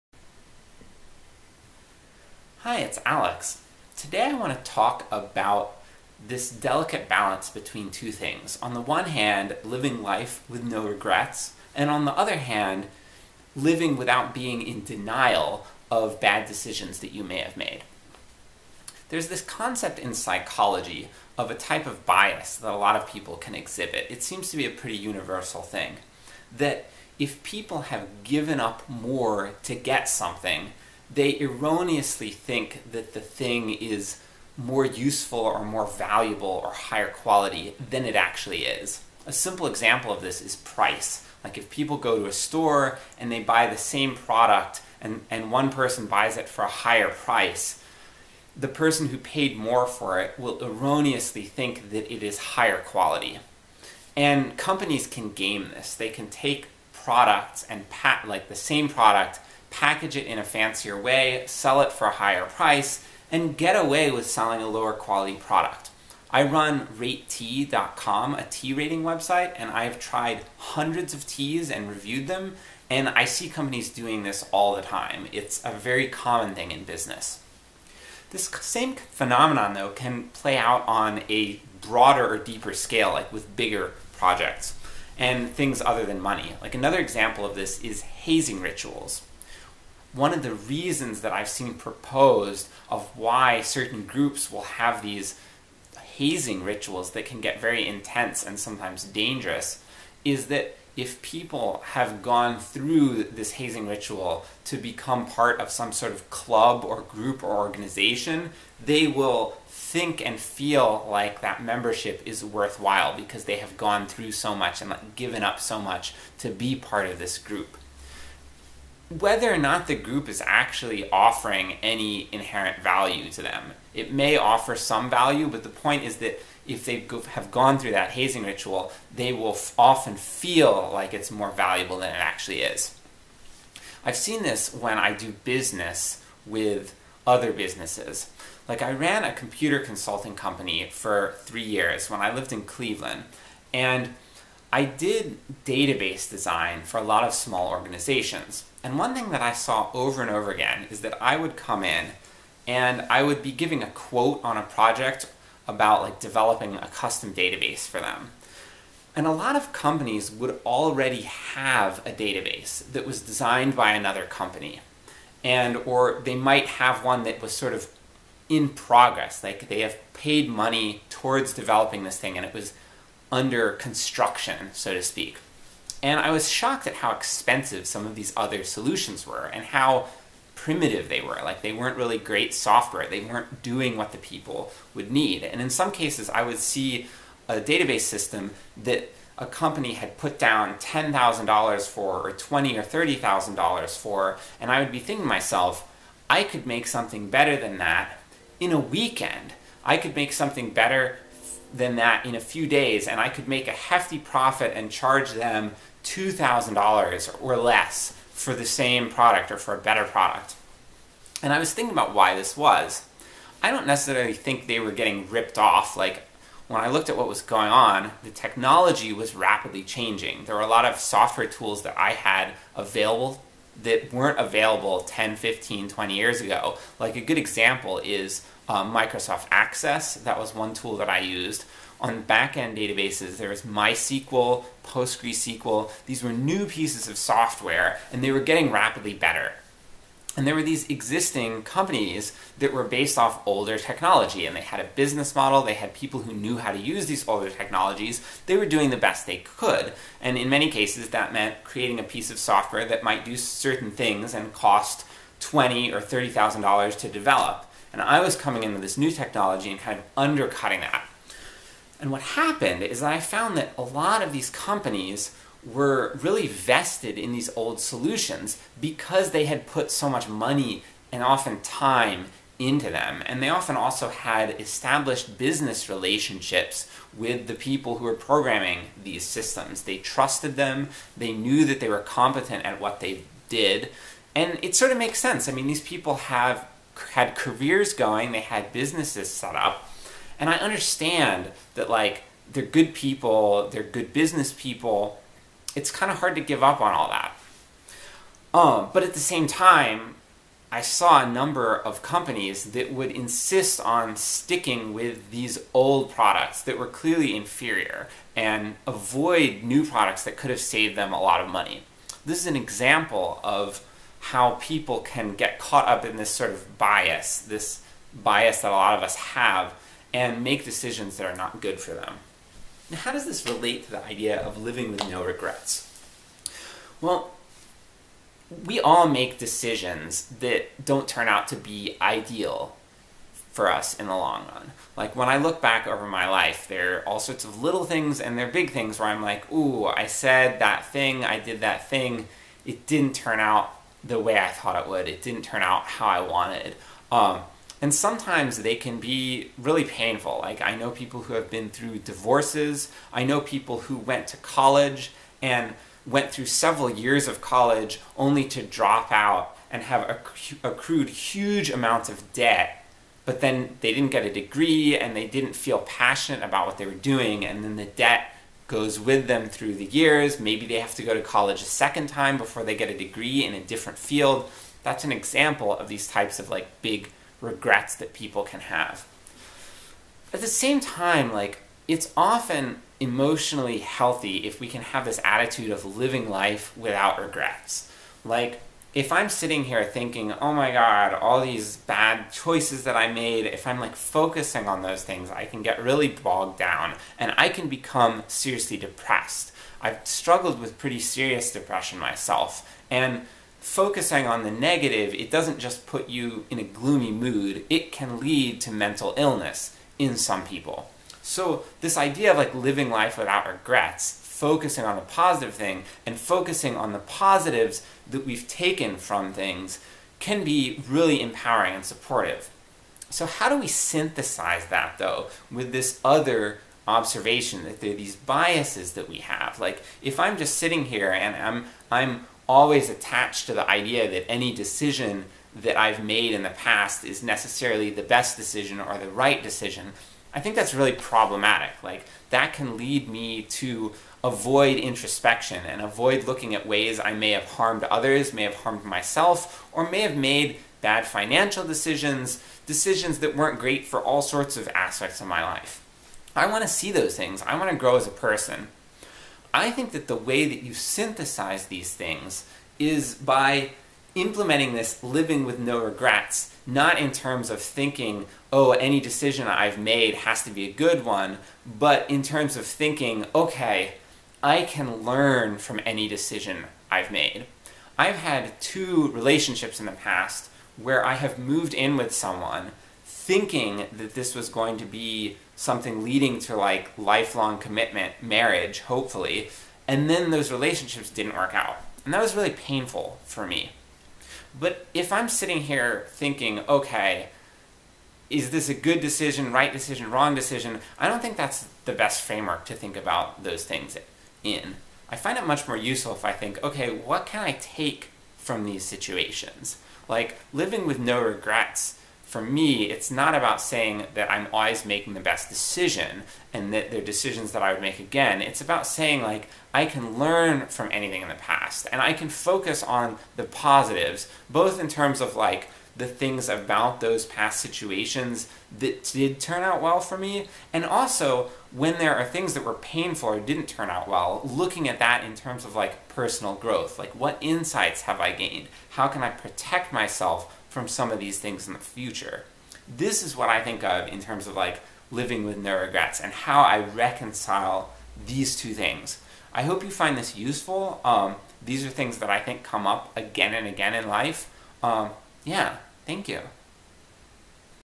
Living with No Regrets, Without Being in Denial
People have a tendency to erroneously think things are more valuable if they've given up more to get them. This can cause people to be in denial about bad choices they made in the past. But at the same time, the idea of living with no regrets can be emotionally healthy, protecting people from depression.  How can we synthesize these two perspectives, protecting ourselves from this inherent bias, while maintaining a positive and empowering outlook on life?